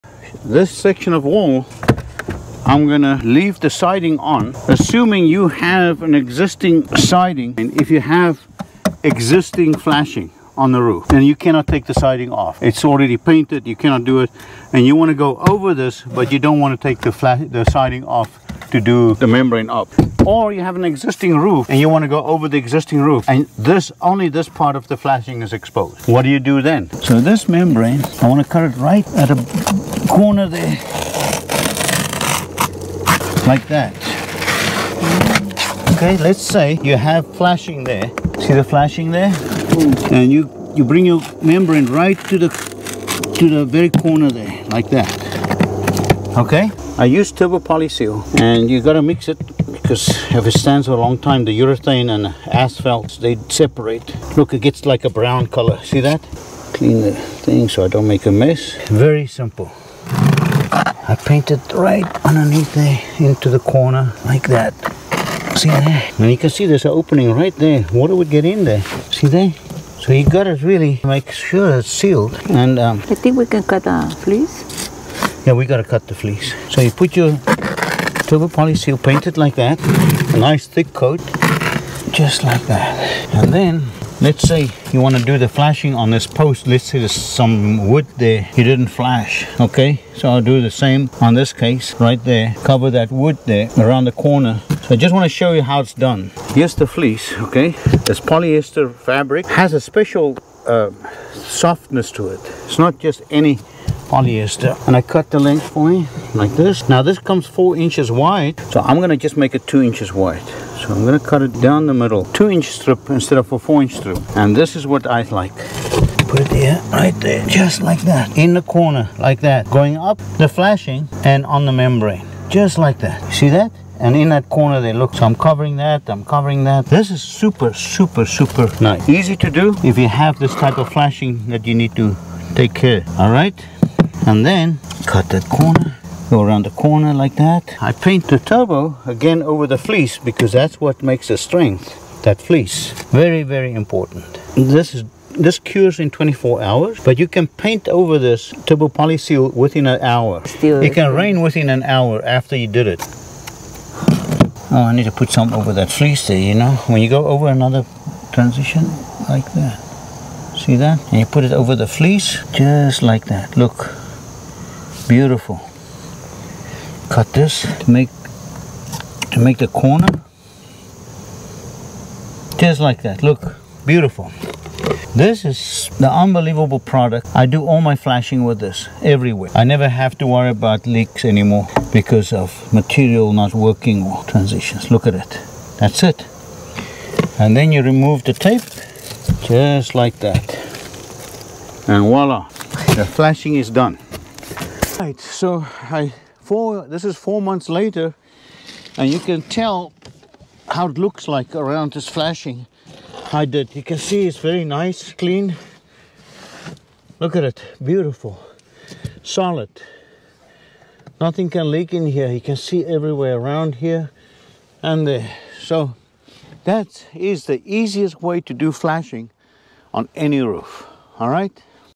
Rubber Flat Roof leak Repair - DIY Liquid Paint on Flashing Repair for Flat Roofs, Vents pipes fl...
How to repair Leaks on Rubber Flat Roofs. Using a brush with Turbo Poly Seal, this DIY Paint on Leak can repair any roof leak or flashing. This repair is simple and will last as long as the roof is there. Anyone can fix leaks on Rubber Roofs or do flashing around vent pipes, chimneys, scuppers, and walls.
Chapters:
0:00 Paint on Leak Repairs
0:47 Turbo Poly Seal for flashing
1:40 Installing the Fleece
3:15 Results after 4 months